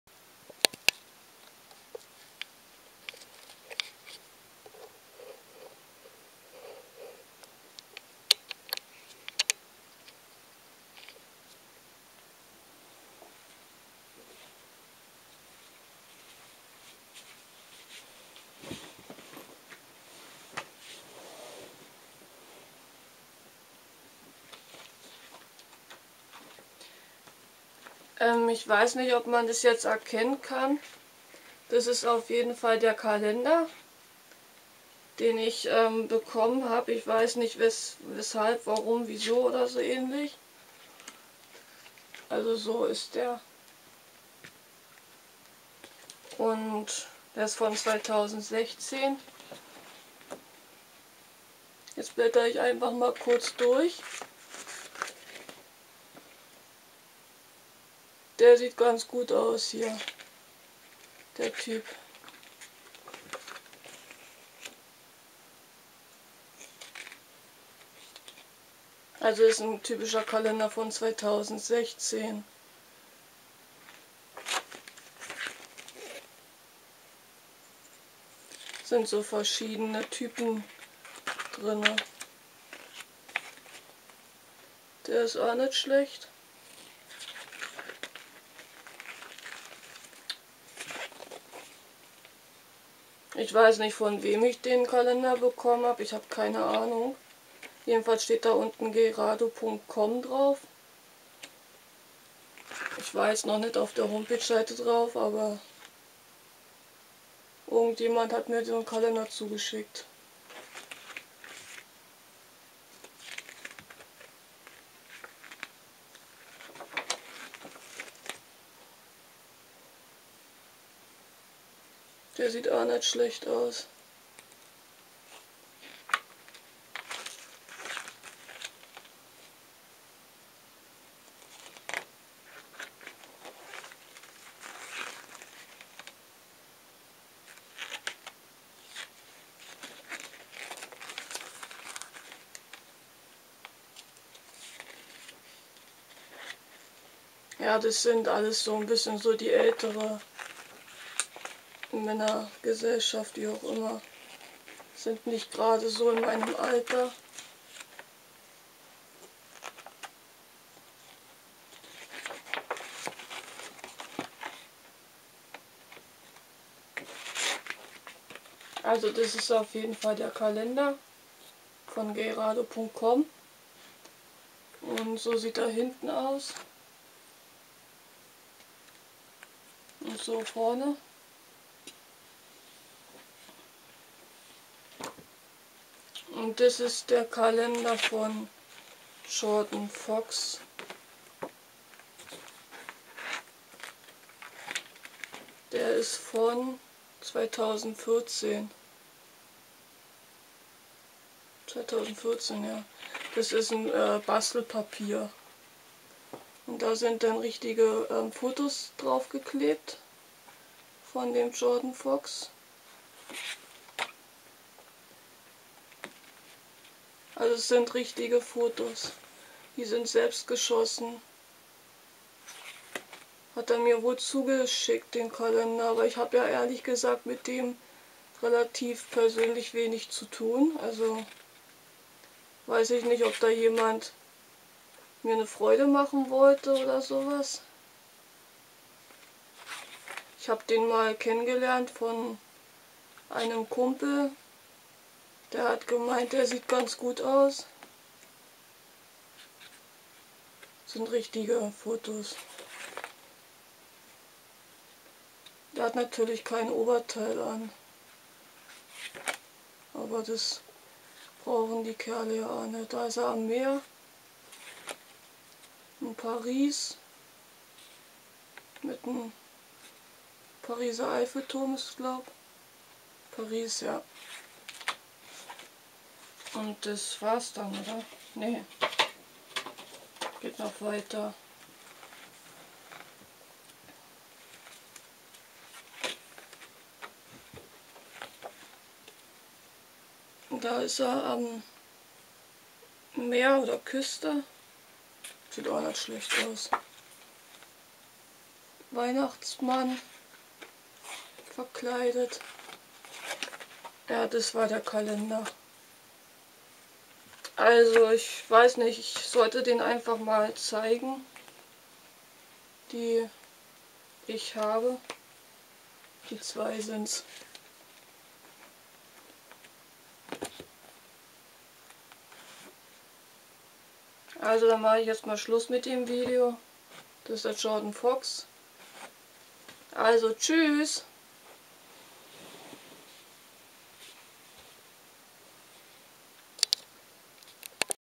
Meine Männer Kalender von 2014 und 2016
Diese zwei Kalender habe ich kostenlos erhalten als Geschenk.
Jordon Fox Kalender und der andere heißt Gayrado.com, wer mir die zu gesandt hat weiß ich nicht aber Danke schön :-D
Sind das nicht süße Männer ? ;-)
Viele Grüße Andrea Piest